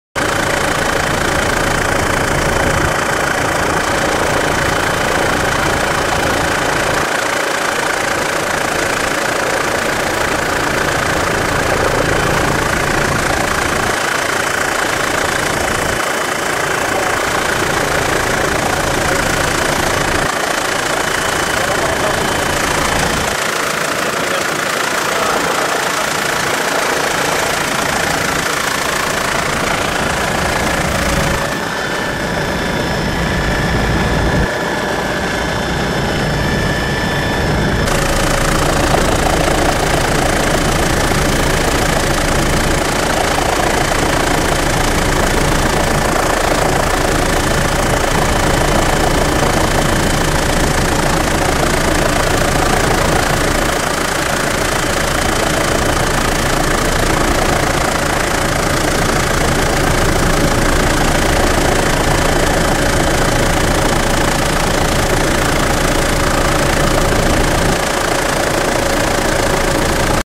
VUELO CIENTÍFICO INVOLCAN - GC 081111 - VOLCÁN SUBMARINO DEL HIERRO - CENIZAS - II
VUELO CIENTÍFICO INVOLCAN - GUARDIA CIVIL HELICÓPTEROS SOBRE LA ZONA DEL VOLCÁN SUBMARINO DE LA RESTINGA, EN EL SUR DEL HIERRO, EN LA MAÑANA DEL DÍA 8 DE NOVIEMBRE DE 2011. PROPIEDAD DEL INSTITUTO VOLCANOLÓGICO DE CANARIAS.